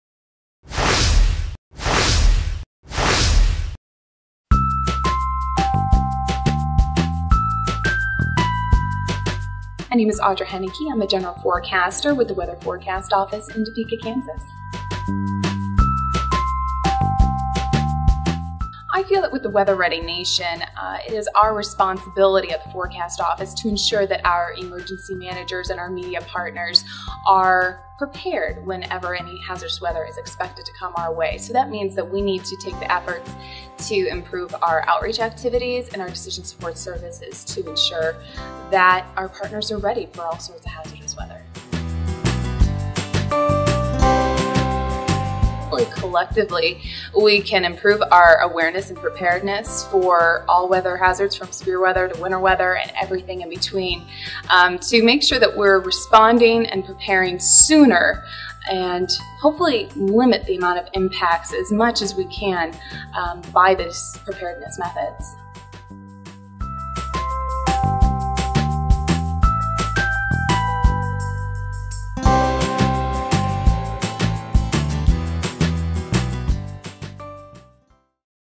WRN: My Role as a Forecaster
In this video, General Forecaster Audra Hennecke discusses her role in the Weather Ready Nation initiative.

National Weather Service Training Center
Kansas City, MO